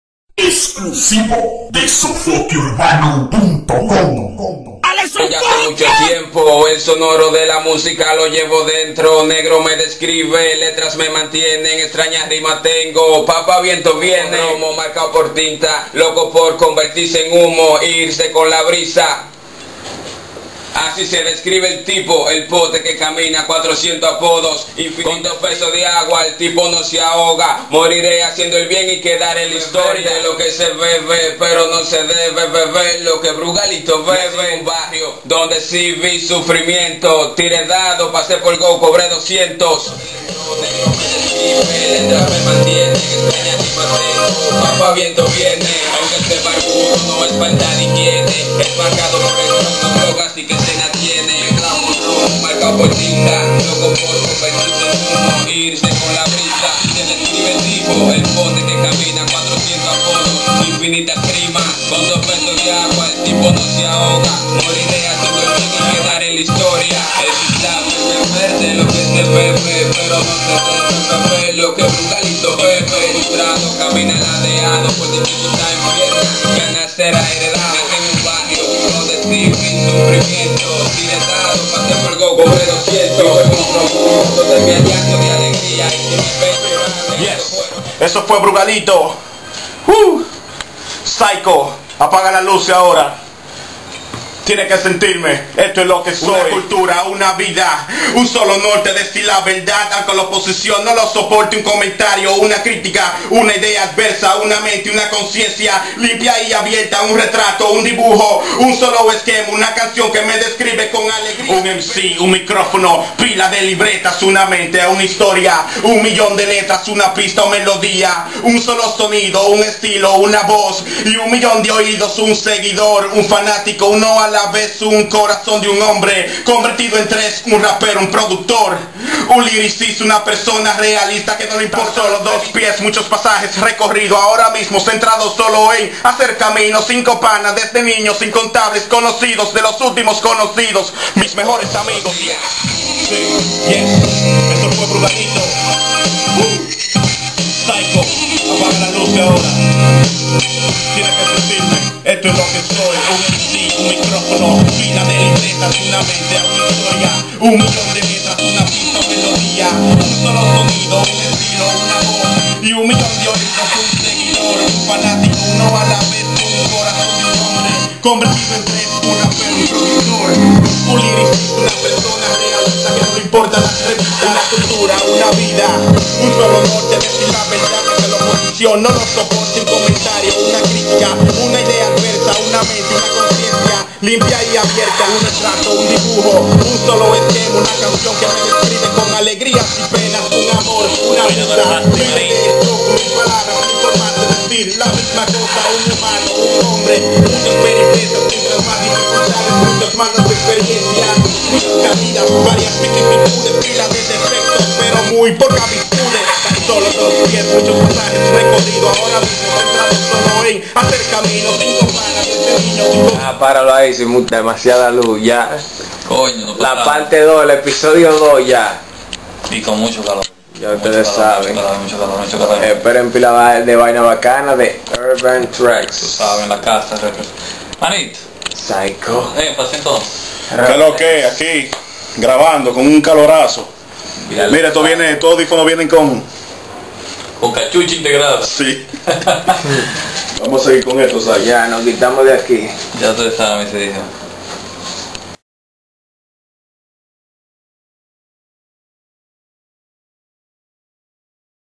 ROLEXBEATZ & BRUGALITO COCINANDO NUEVO TEMA Y ENTREVISTA A PSYCHO PARTE 2
URBAN TRACKZ